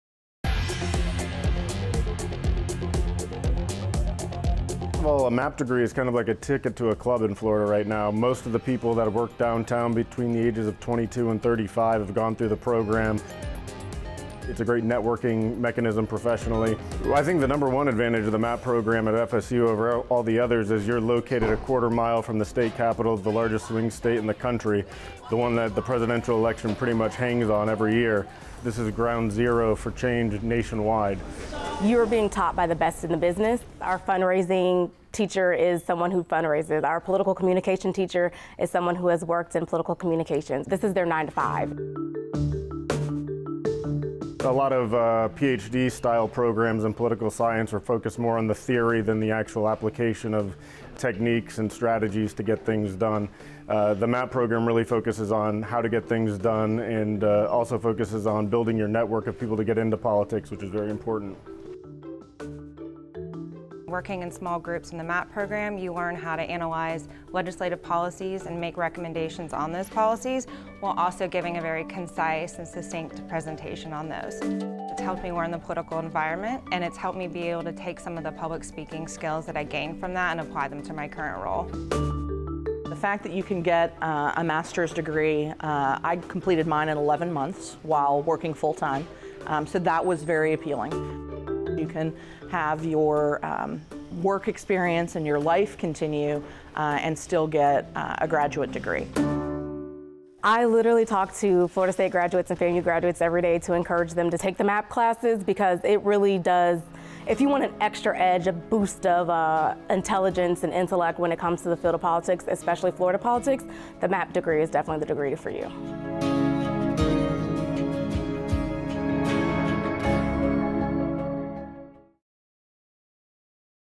FSU Master's of Applied American Politics & Policy
The MAAPP program is a terminal Masters of Science degree designed for those seeking active careers in a number of political fields including campaigns, fundraising, political communications, lobbying, grassroots organizing and legislative affairs.

Unlike the traditional graduate program in Political Science, which is designed for individuals with career paths in academia and research, MAAPP offers practical experience and a diverse set of skills for those interested in careers as political operatives. Complete your degree in one year of full-time study. Hear more from alumni of the program.